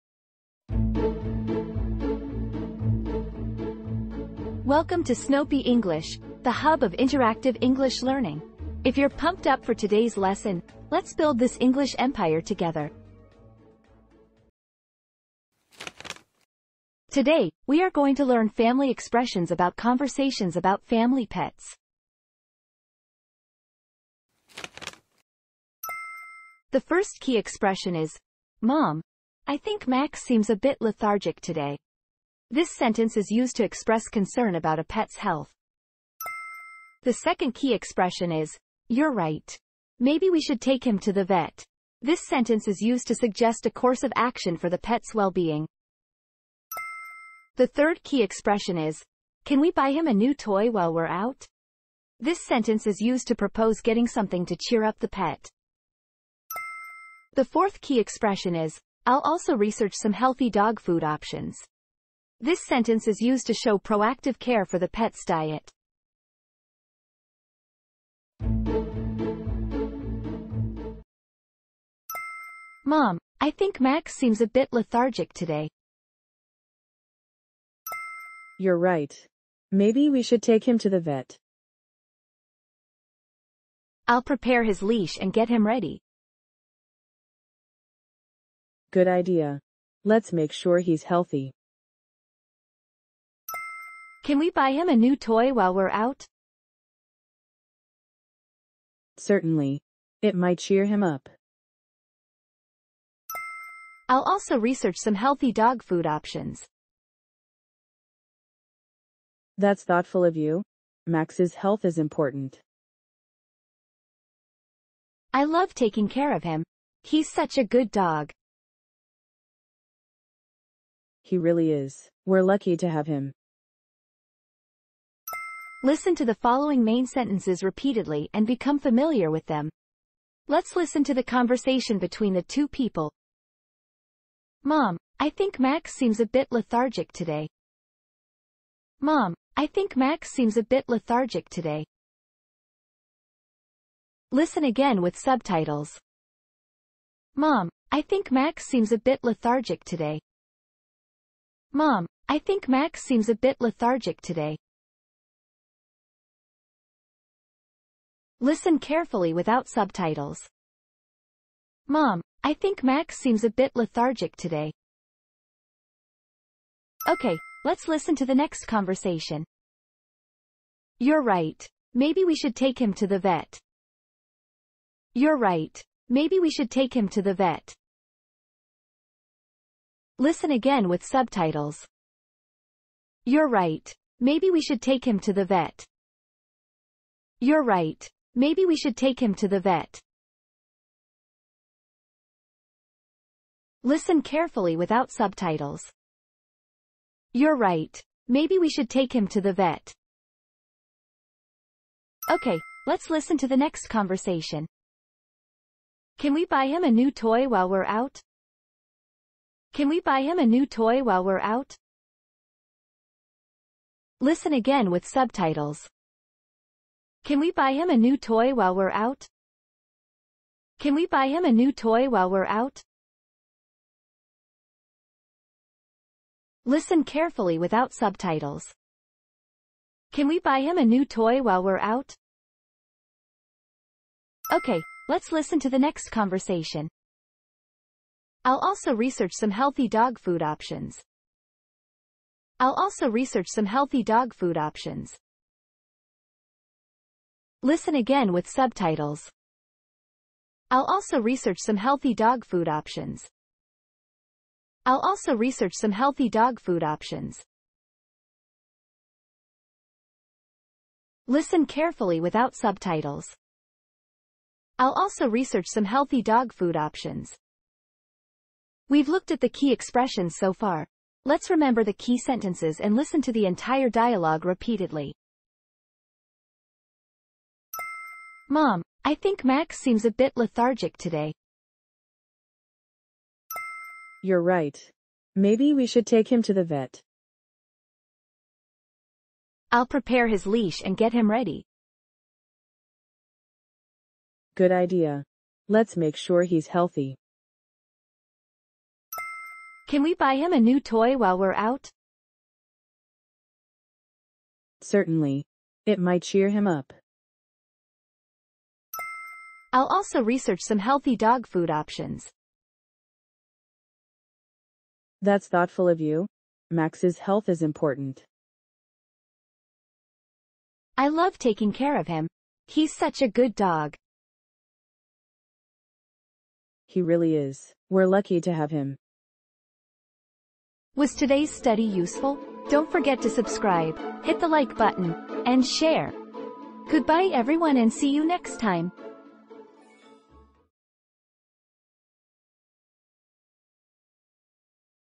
Learn English | Family | Family Pets - Snowpea English
Do you want to learn English conversations about family?
English teacher Jenny will guide you through essential conversations about family matters.
Learn useful English expressions with "Snowpea English"
Master English through repetitive listening and speaking with Snowpea English Videos.